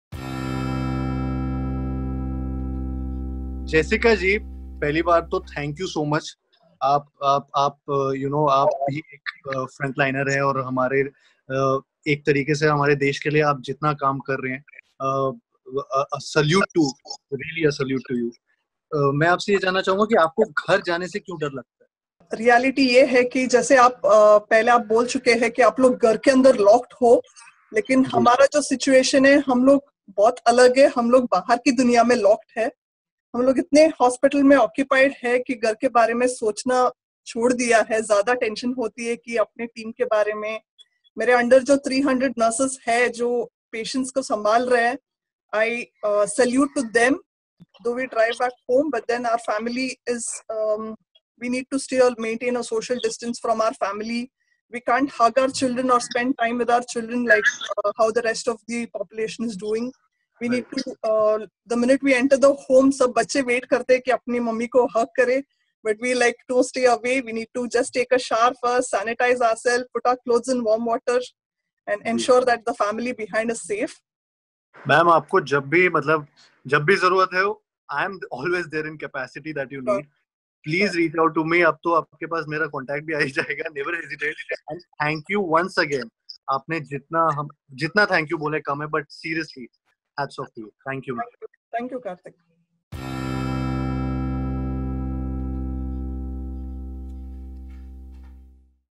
Watch Kartik Aaryan talk with Jessica DSouza, Chief Nursing Officer, Global Hospital, Mumbai
Watch Kartik Aaryan talk with Covid-19 Hero Jessica DSouza, Chief Nursing Officer, Global Hospital, Mumbai. Jessica tells Kartik how she supervises 300 nurses handling Covid-19 patients. Listen to her first hand as she takes us through her day. MyyIndia salutes Jessica. You're doing an amazing job.